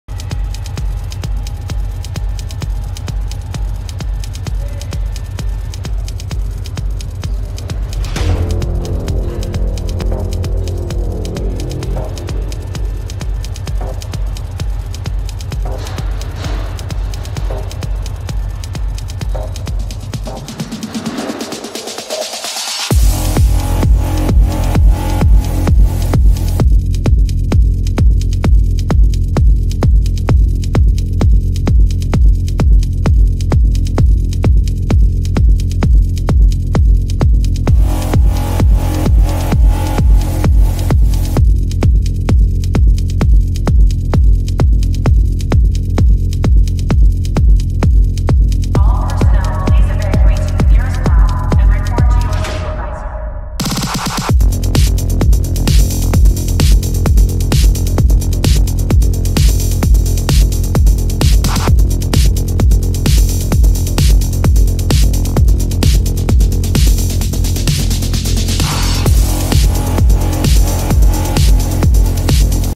Zbrush 2019 медик обзор
Медик. 50 лет. Медик это высоко образованный немецкий полевой доктор. У него высшее медицинское образование, но он лишился врачебной лицензии, изобрел лечащее орудие и чтобы проверить пошел в тимфортресс! Очень галантен...